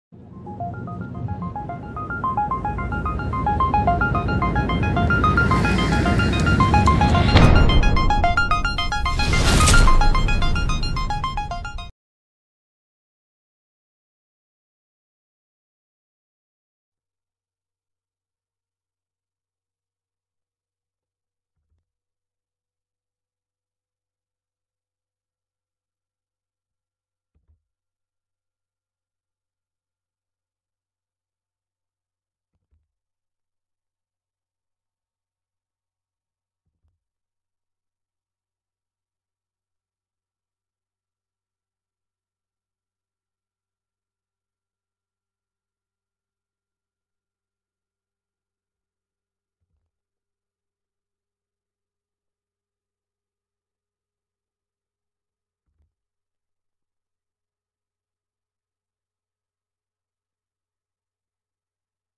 getexture prototyper videotutorial
This short videotutorial shows how to use geTexture, a small software Arc-Team developed to use the Soil Texture Triangle during archaeological excavations.